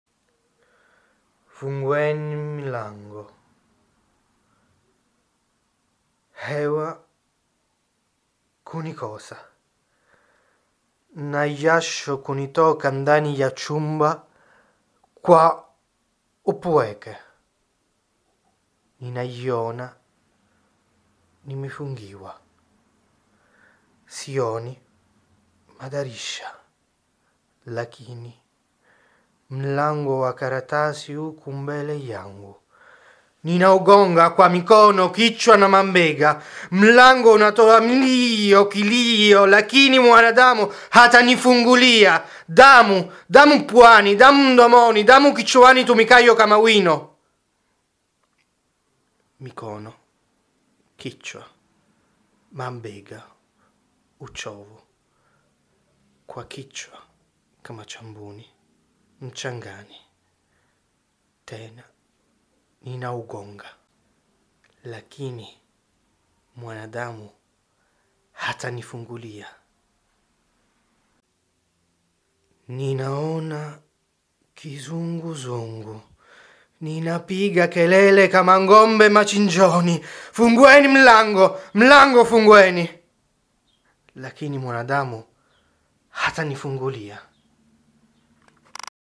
Fungueni mlango - Euphrase Kezilahabi
Shairi la "Fungueni mlango" (diwani ya Kichomi 1974) la Euphrase Kezilahabi (Ukerewe 1944 - Dar es Salaam 2020).

Hewa kunikosa
Na jasho kunitoka ndani ya chumba
Kwa upweke
Ninajiona nimefungiwa.
Sioni madirisha lakini
Mlango wa karatasi uko mbele yangu
Ninaugonga kwa mikono
Kichwa na mabega
Mlango unatoa mlio kilio,
Lakini mwanadamu hatanifungulia.
              Damu
Damu puani, damu mdomoni,
Damu kichwani itumikayo kama wino.
Mikono, kichwa, mabega uchovu.
Kwa kichwa kama cha mbuni
Mchangani, tena ninaugonga
Lakini mwanadamu hatanifungulia.

Ninaona kizunguzungu
Ninapiga kelele kama
Ng’ombe machinjoni:
Fungueni mlango!
Mlango fungueni!
Lakini mwanadamu hatanifungulia.